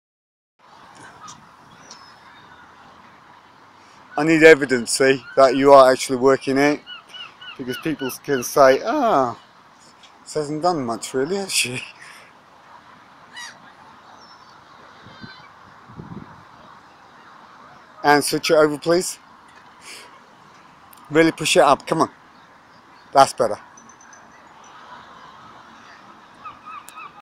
Personal Training
1-2-1 Personal training with individuals. Helping them to achieve their goals whatever they may be. Health, Fitness, Weight lose, weight Gain, sports specific. Bodybuilding.
Long distance running, Speed running, Cycling and more.
why train indoors when you can train at great locations.
 Here I Am Outdoors training with one of my clients at Clacton on On Sea